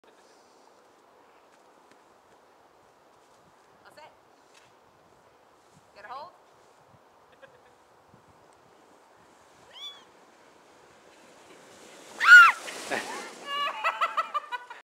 Lori takes a slide down the hill
Lori takes a slide down the snow hill on a cardboard sled.